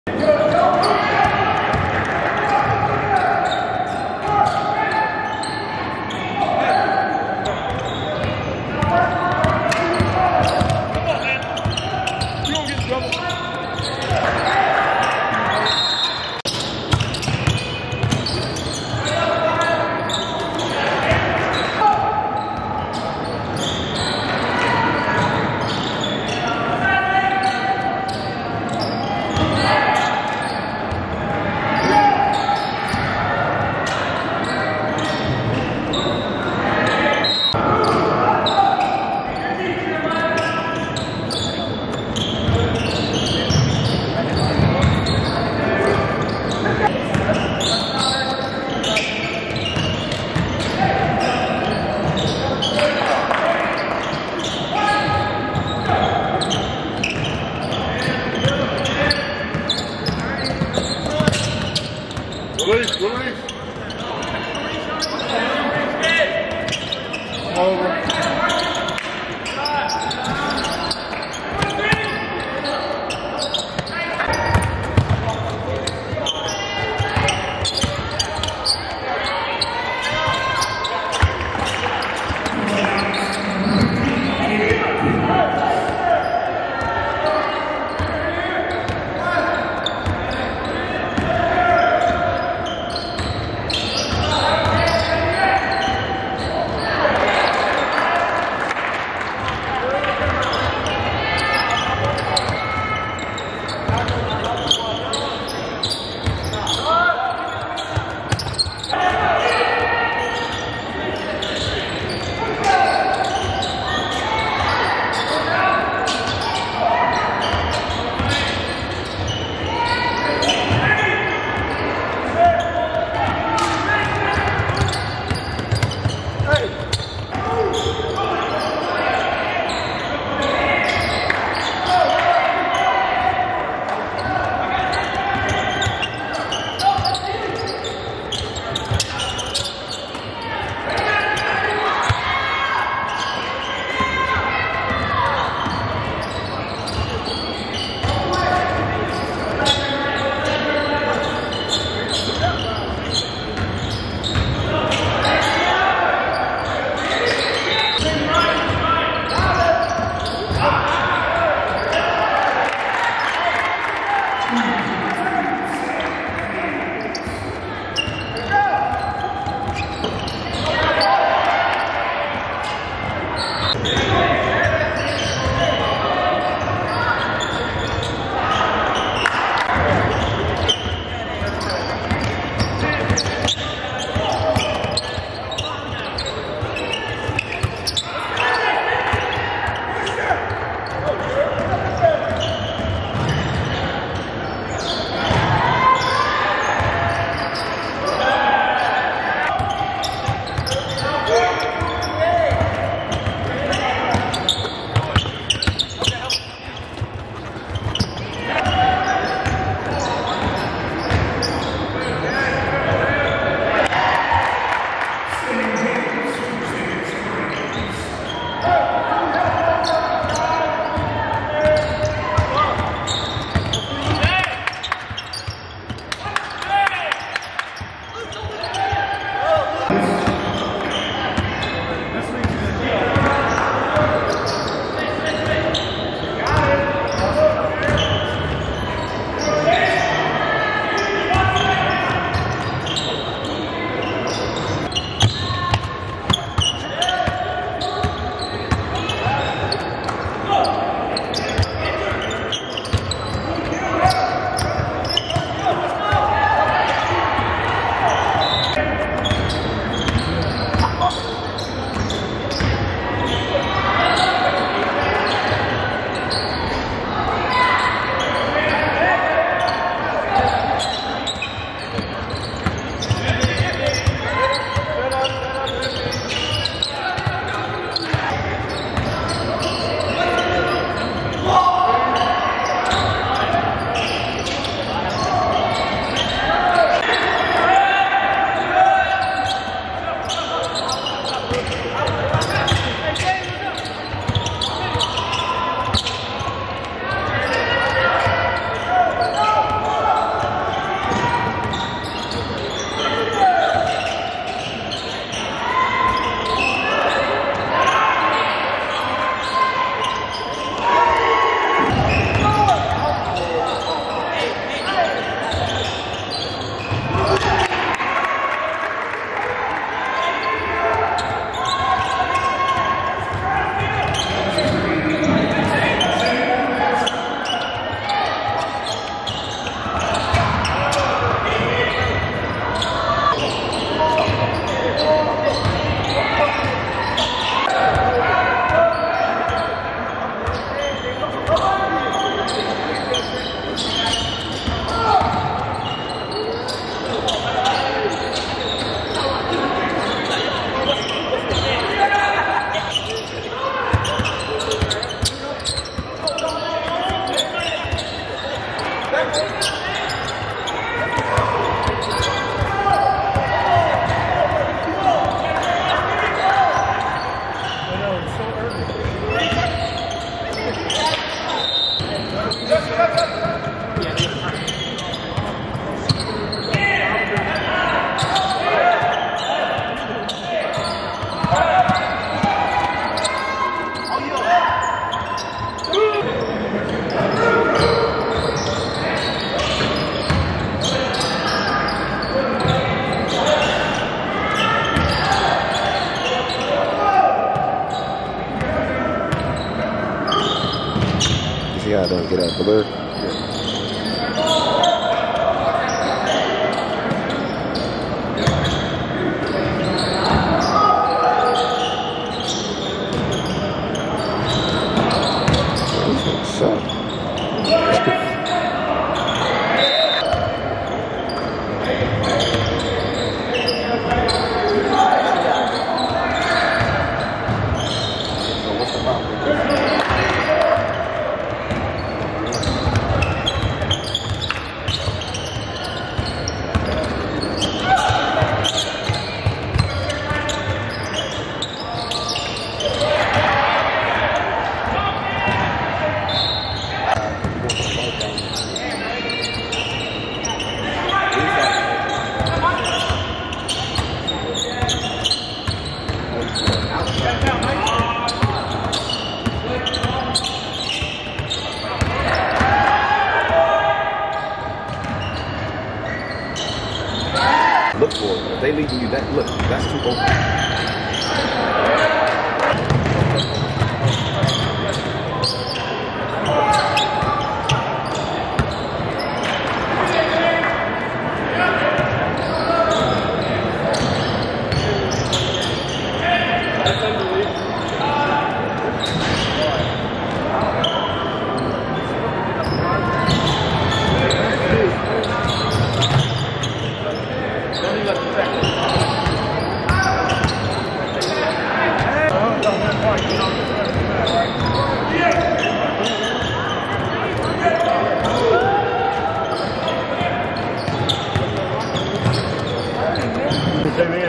Sand Springs VS OKC Knights | Tulsa Memorial High School tournament.
This is round one of Tulsa Memorial High School tournament. This is OKC Knights VS Sand Springs.